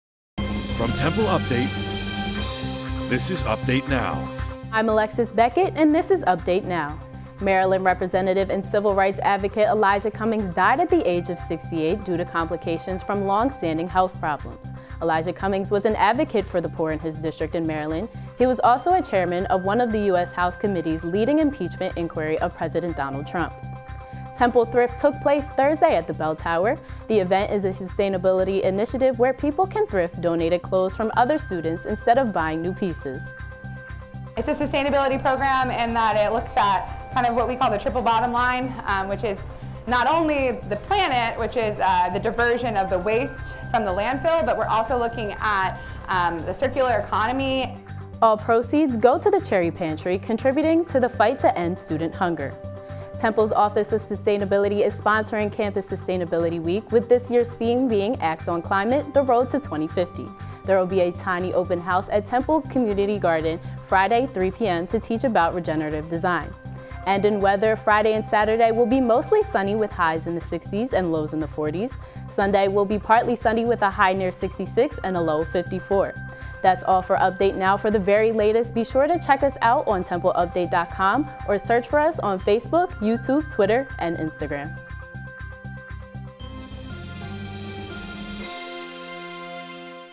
Update Now || October 17, 2019
From the Temple Update Newsroom, This is Update Now. Here's what you need to know:

- Elijah Cummings Dead
- Temple Thrift
- Sustainability Week